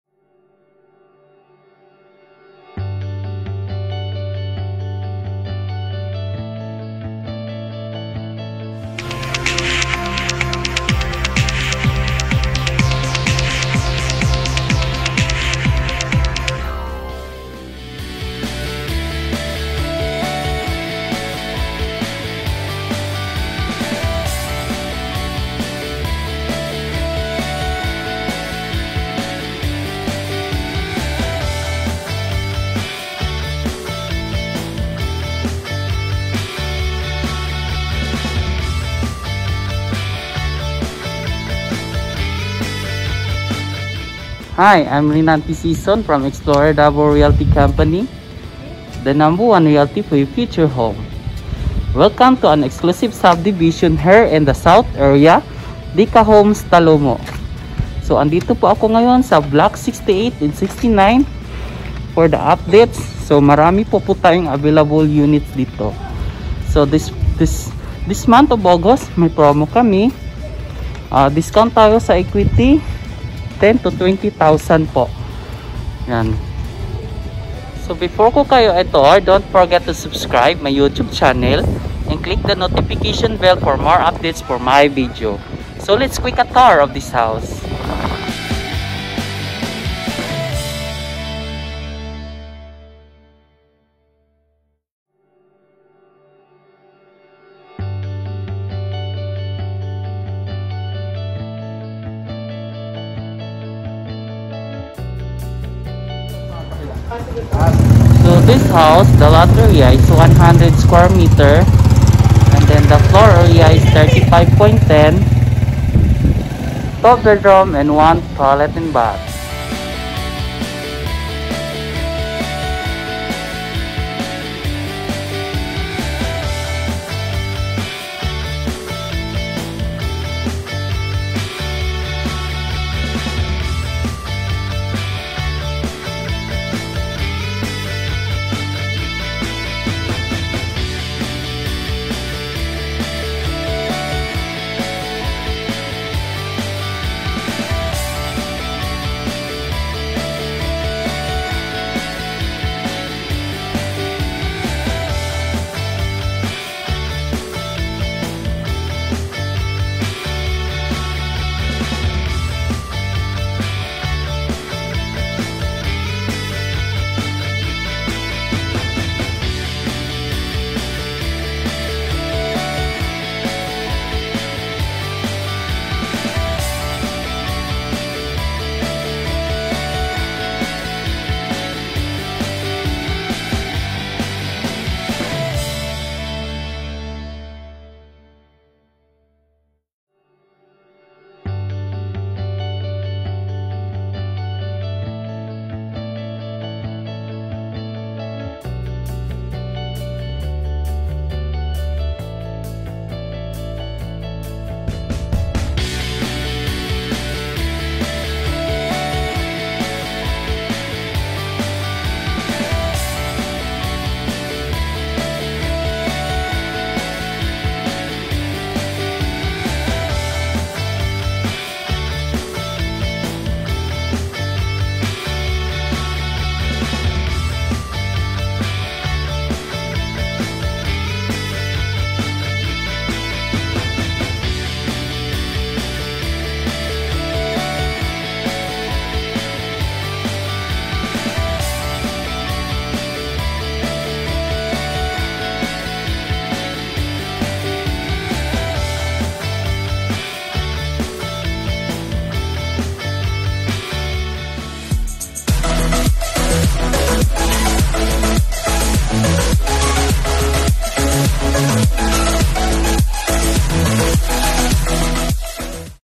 BLOCK 68 AND 69 IN DECA TALOMO UPDATE | RENT TO OWN UNIT
Deca Homes Talomo is located in Libby Road Bago Gallerra Davao city.

Avail our Rent to Own Unit in Deca Homes Talomo.

Bungalow Unit with 2 Bedroom and 1 Toilet and bath.
The lowest Downpayment in housing davao city.